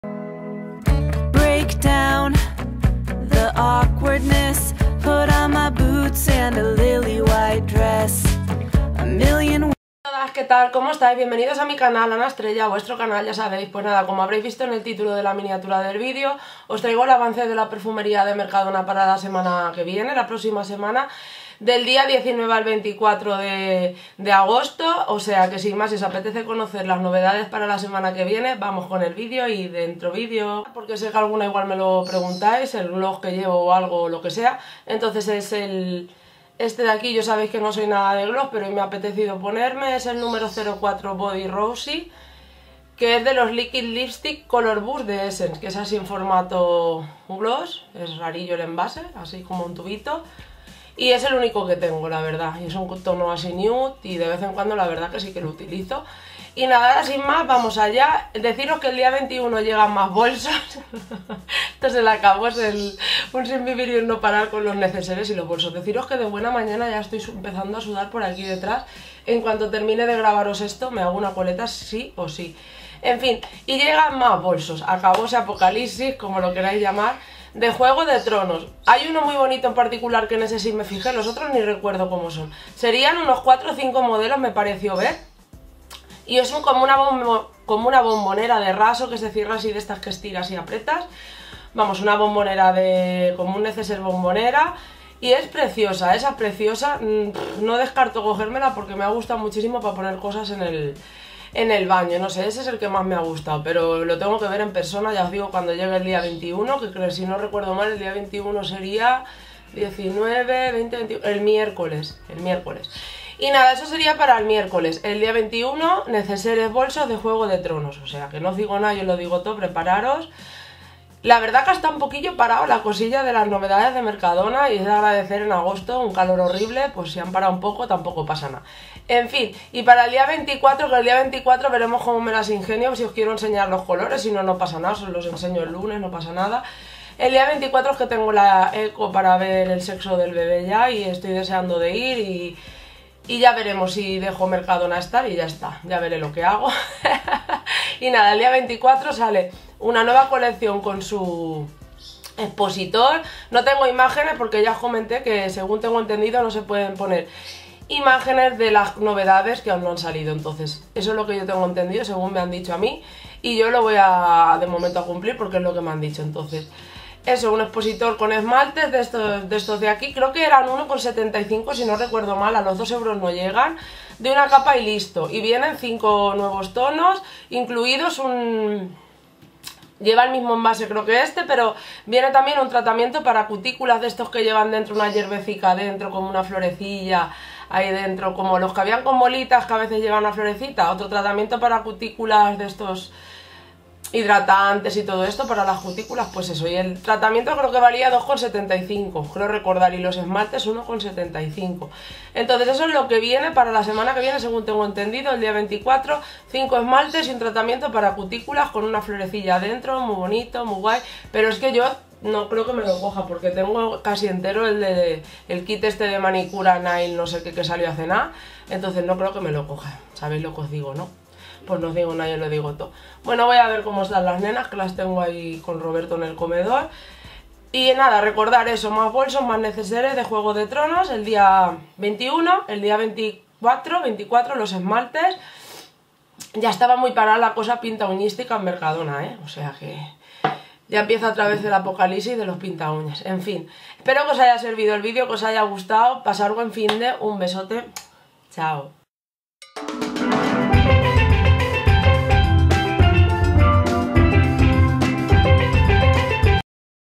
😱AVANCE NOVEDADES🔥PERFUMERÍA DE MERCADONA💣¡AÚN NO EN VENTA!💥AGOSTO DEL 19 AL 24
-HOLA!NUEVO VIDEO CON EL AVANCE DE LAS NOVEDADES DE LA PERFUMERÍA DE MERCADONA PARA LA PRÓXIMA SEMANA DEL 19 DE AGOSTO AL 24!¡NOVEDADES AÚN NO A LA VENTA EXCLUSIVAS!